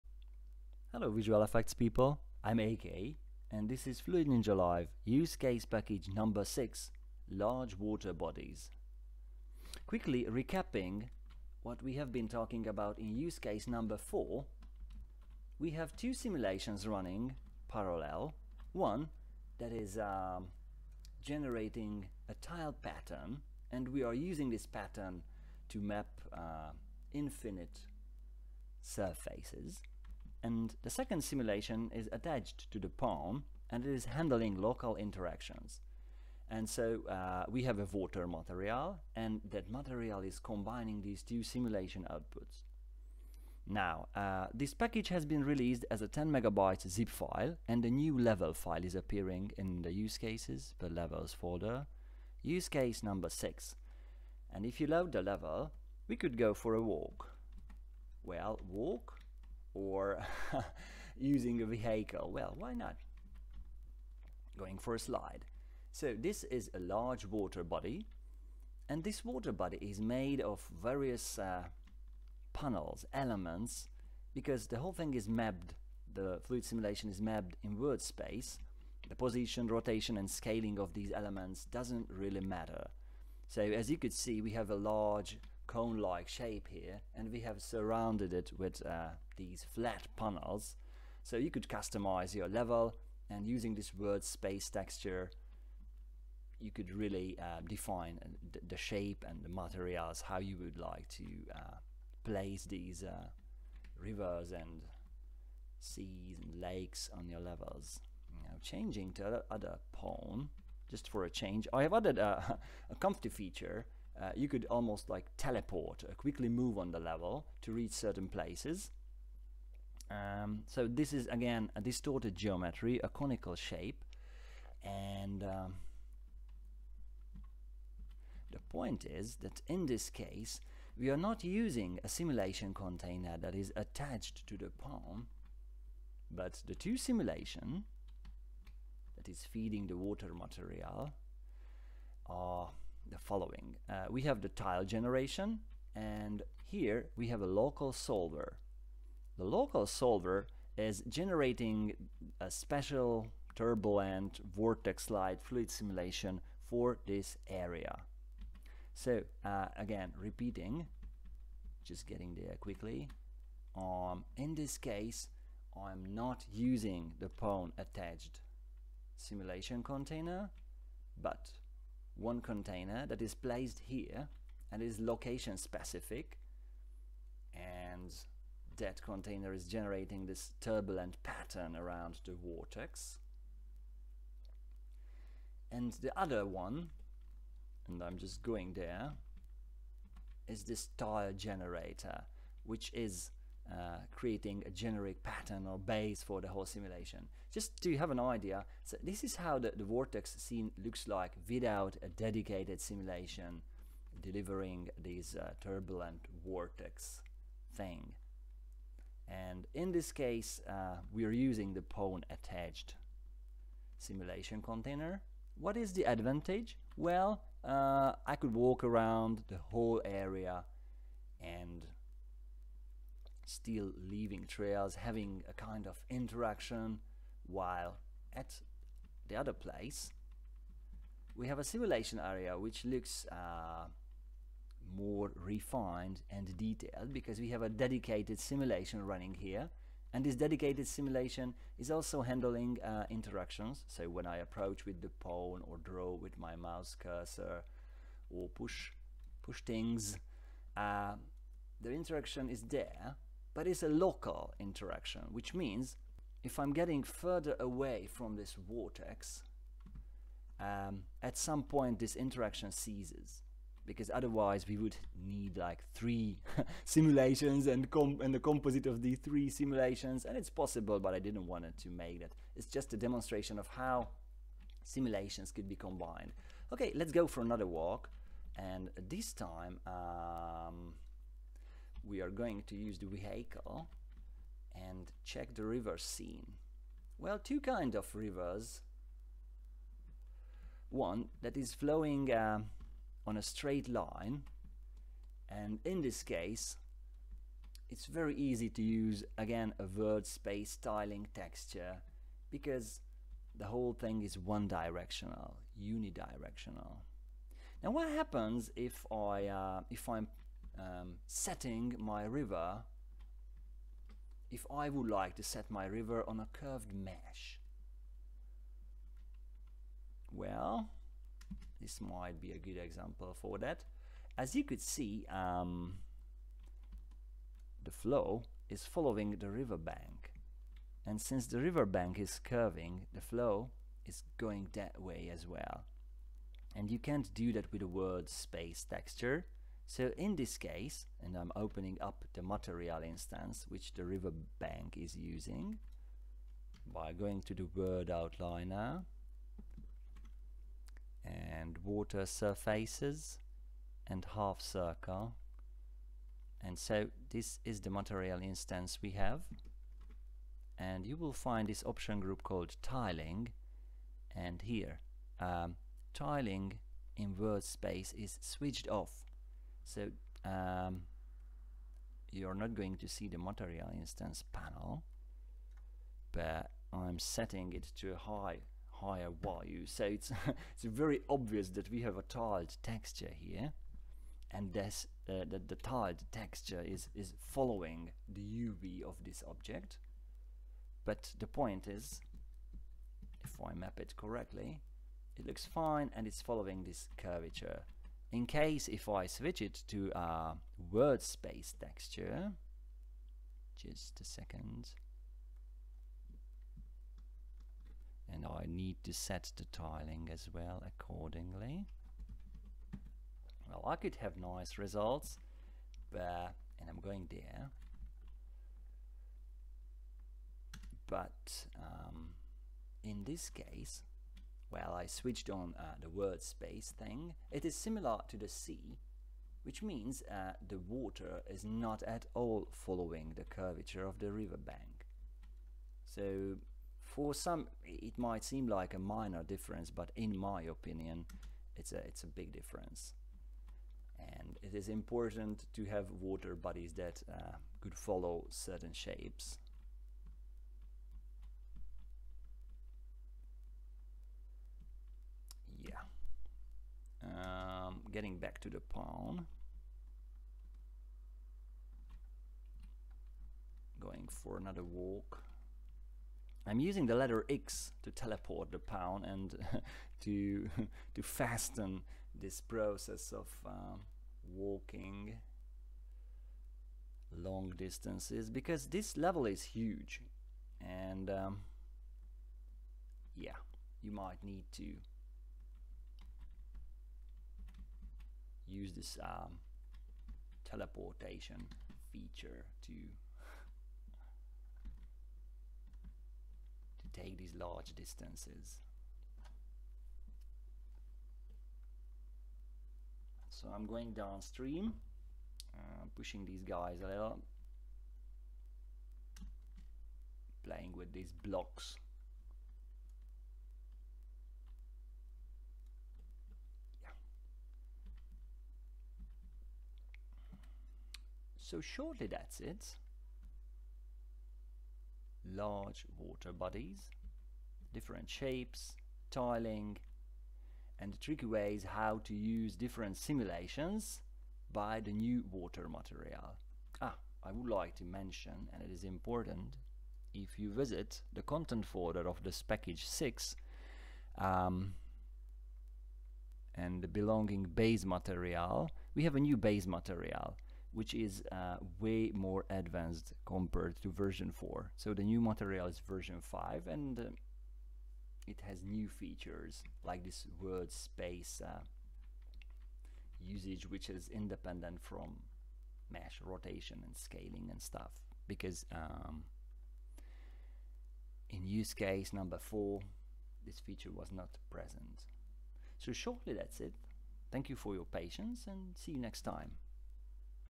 FluidNinja LIVE - UseCase 6 TUTORIAL - Rivers, Lakes, Whirlpools (OUTDATED)
The addon package demonstrates how multiple ninja simulations are composited together to create large water bodies and rivers.